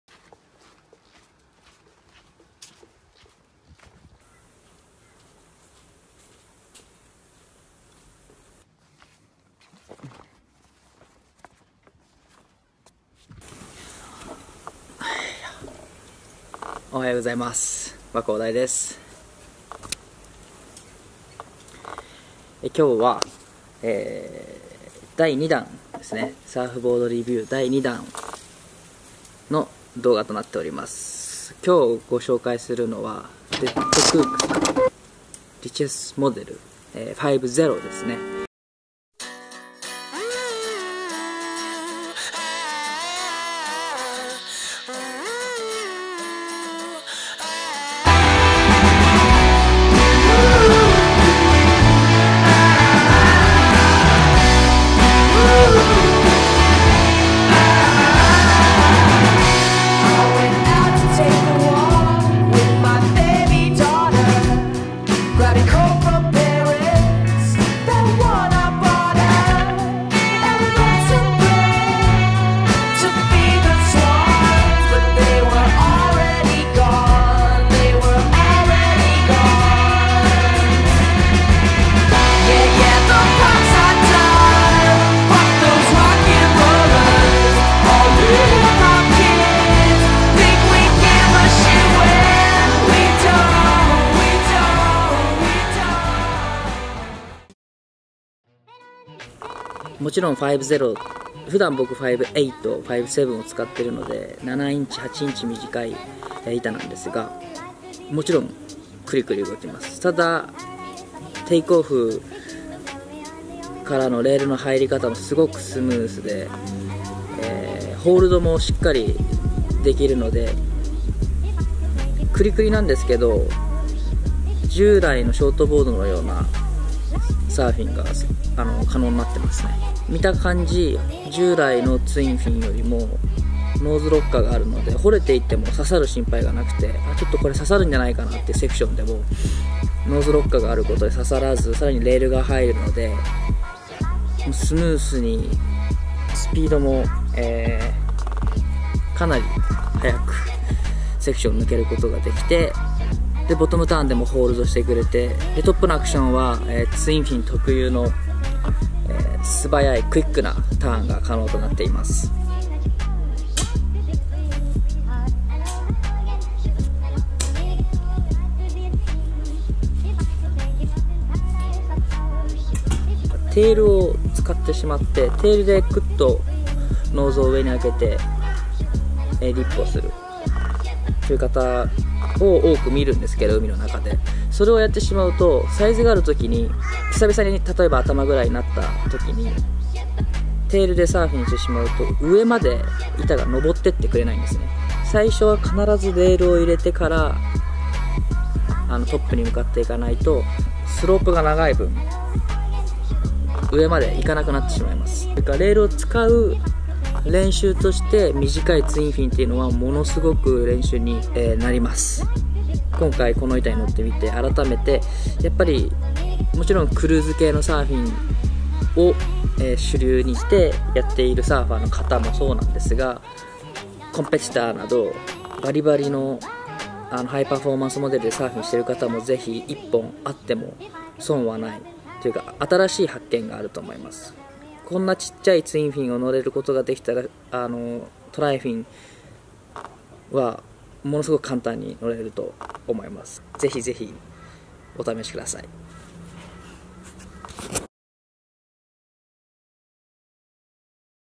【Surfboard Review #2 DEADKOOKS 5'0 RICHES】Life of Wombat -Dai Wako
第二弾目のサーフボードレビュー動画！
今回ご紹介するのはDEADKOOKS のRICHES モデル。クラシカルなアウトラインにオシャンティーのフィン。ノーズロッカーも若干あるので掘れている波も刺さらずレールも入れやすく、大きな波も小さな波も幅広く使えるセットアップとなっています。
サーフィン技術向上のためにも一家に一台持っていて間違いないんではないんでしょうか！
Reviewing my first Twinny from the world famous shaper Hayden.